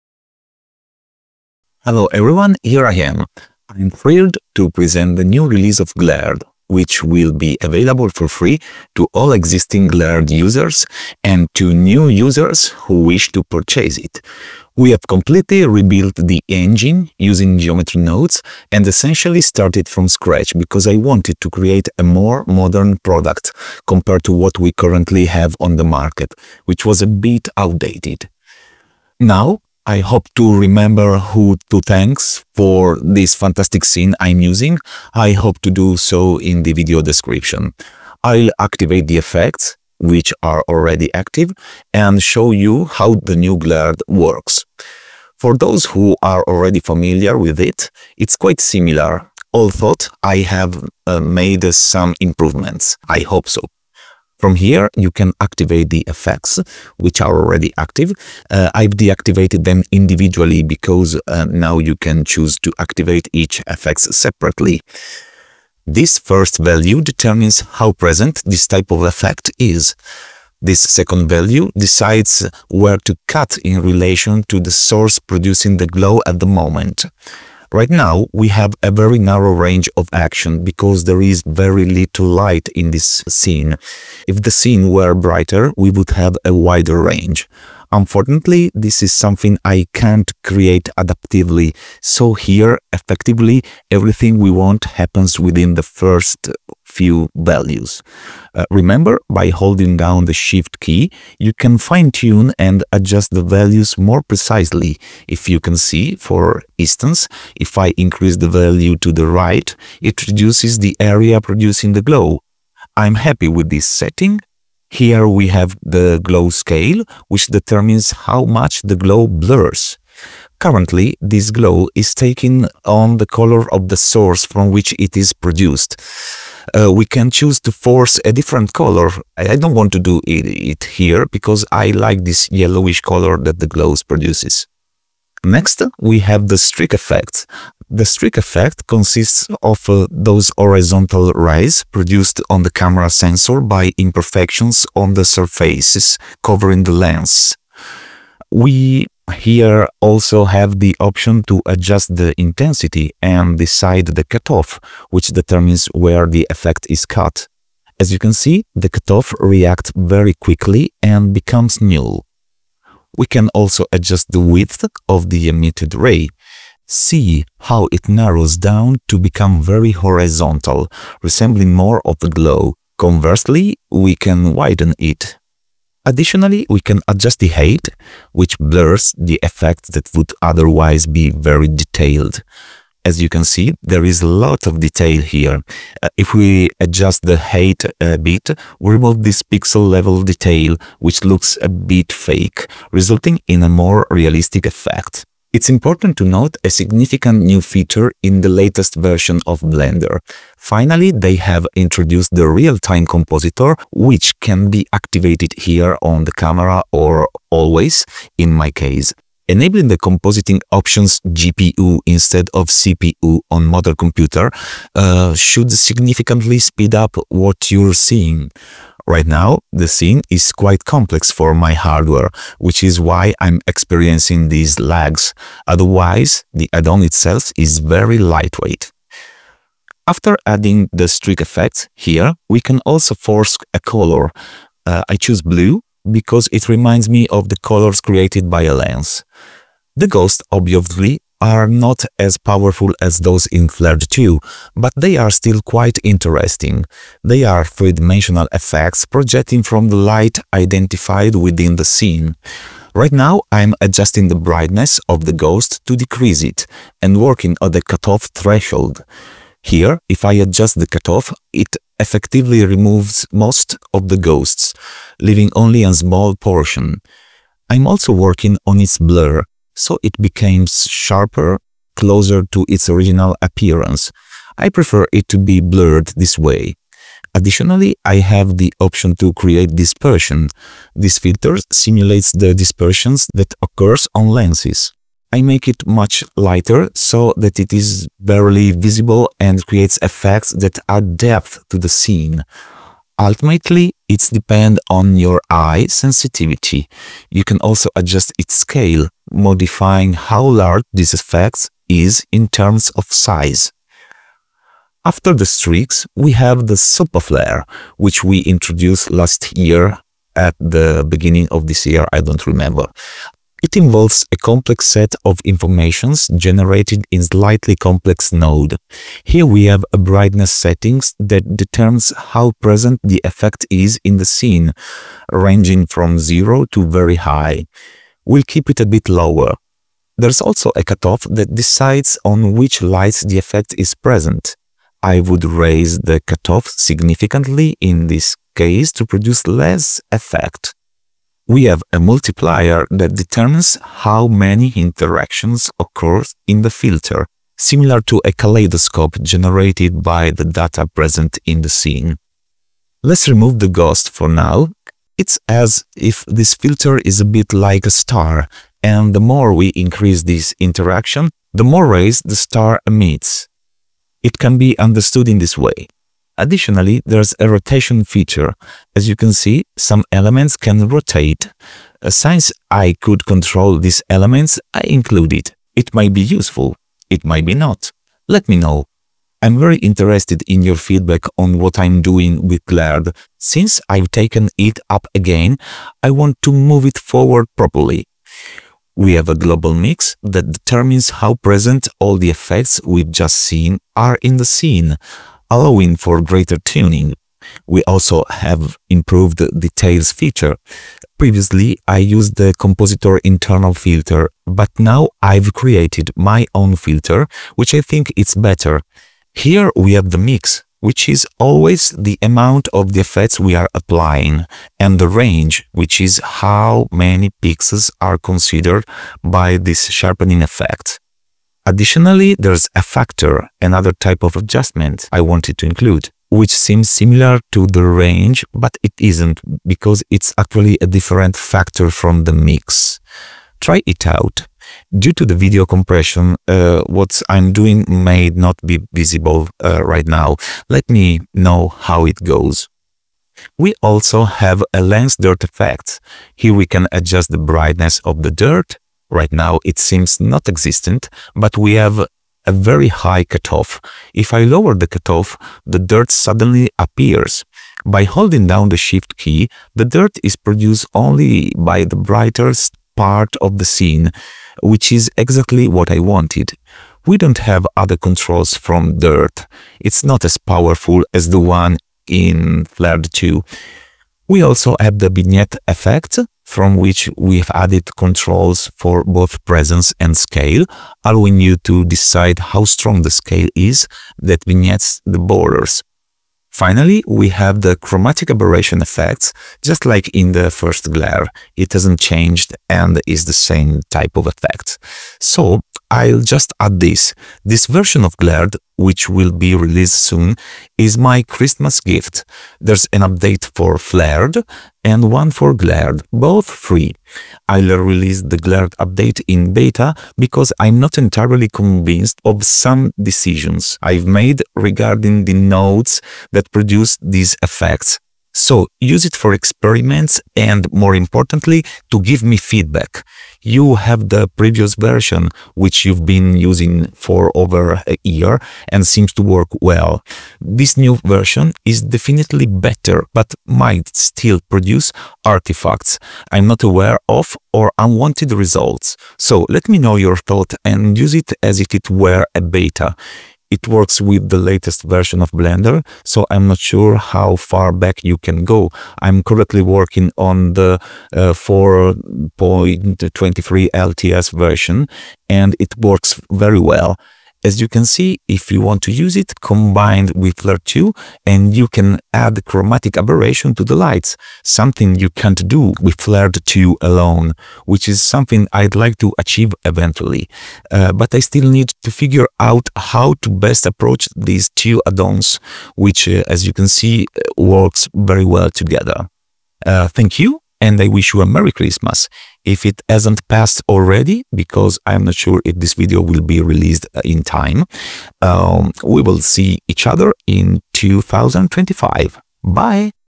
Glared - Massive Free Update for Blender's Ultimate Lighting and Lens Flare Addon
Introducing the massive free update for Glared, the premier lighting and lens flare addon for Blender! Glared has been enhancing Blender projects for over two years, providing stunning screen space-based effects that elevate the atmosphere of your renders and animations.

This upcoming update, set to release in beta in just a few days, is a complete rebuild of the addon from the ground up. Utilizing geometry nodes, the new Glared offers advanced features, an enhanced visual interface, and improved performance. Whether you're a current user or a new buyer, this update is available to everyone with a Glared license on Blender Market.